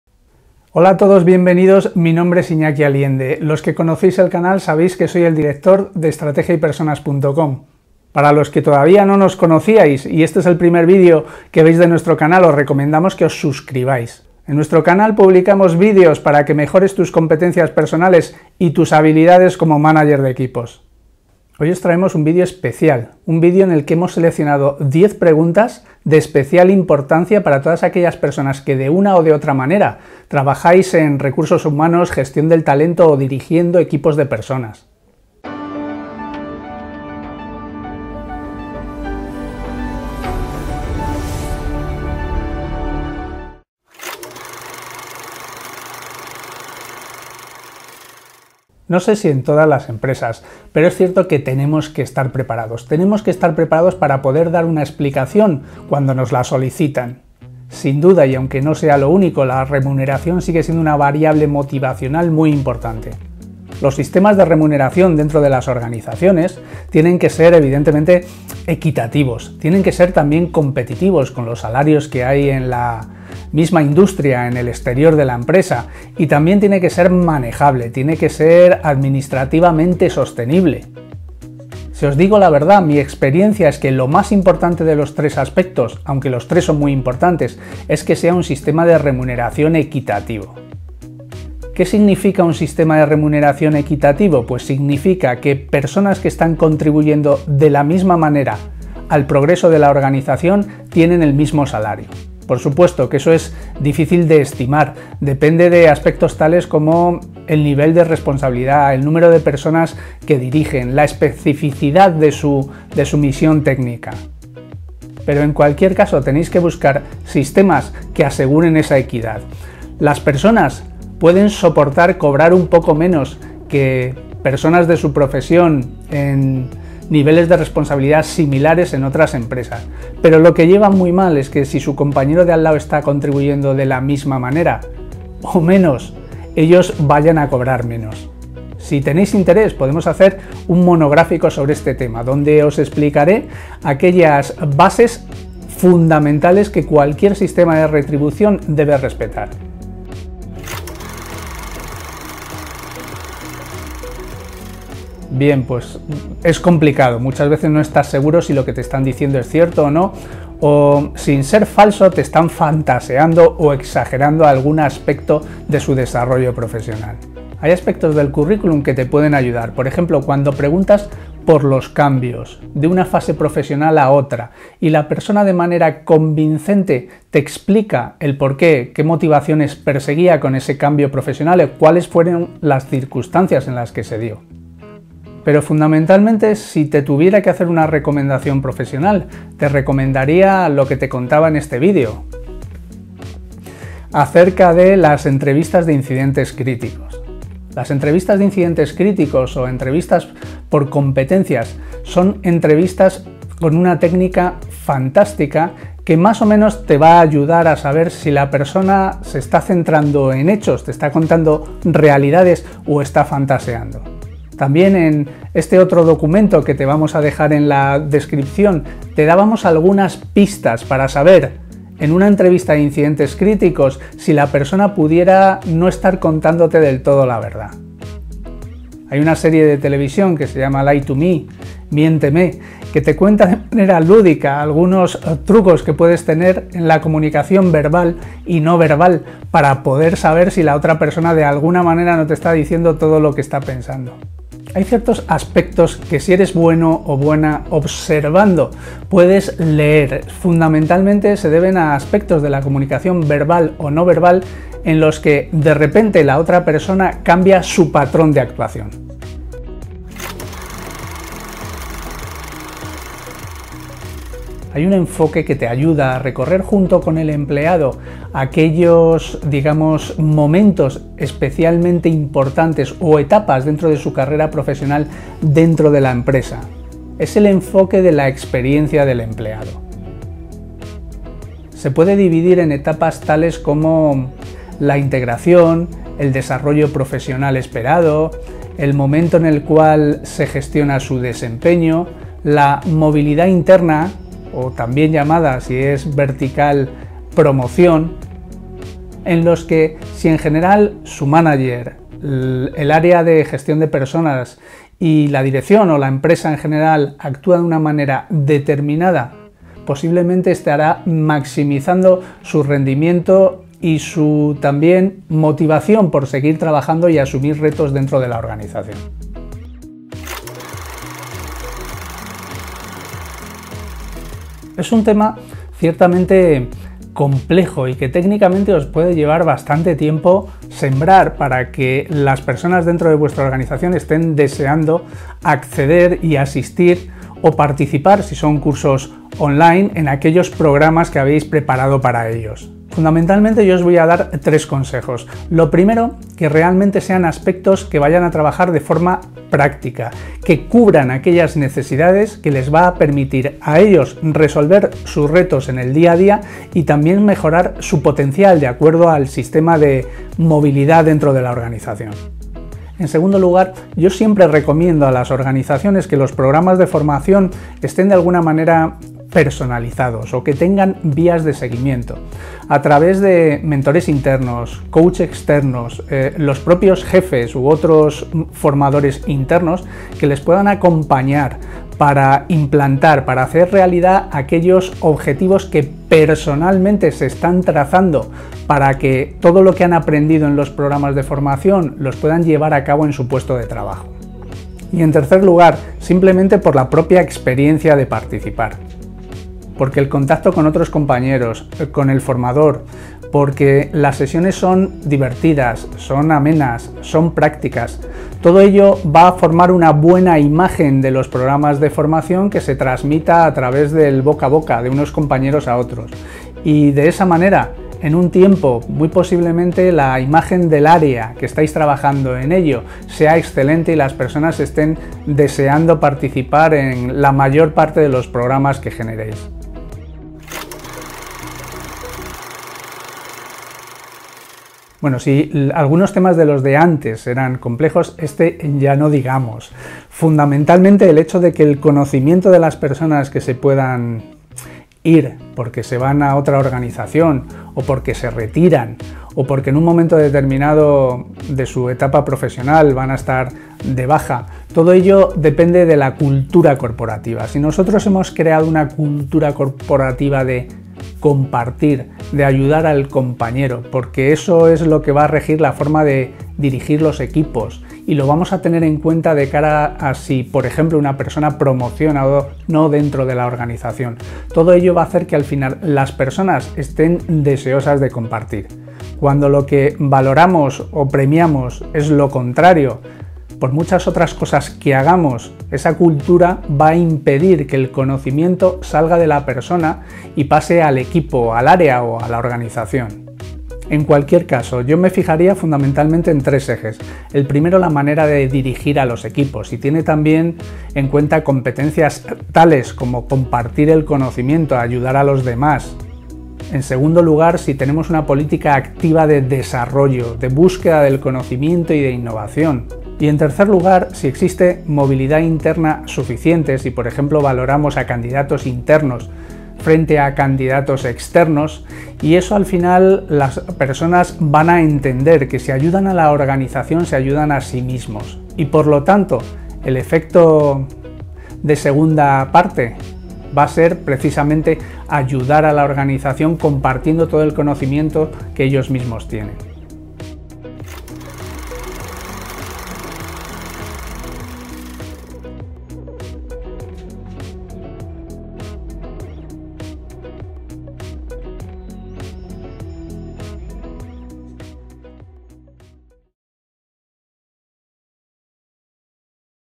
Diez preguntas clave sobre gestión de personas
Iñaki Aliende, director de responde las preguntas más controvertidas sobre la gestión de personas y Recursos Humanos en las empresas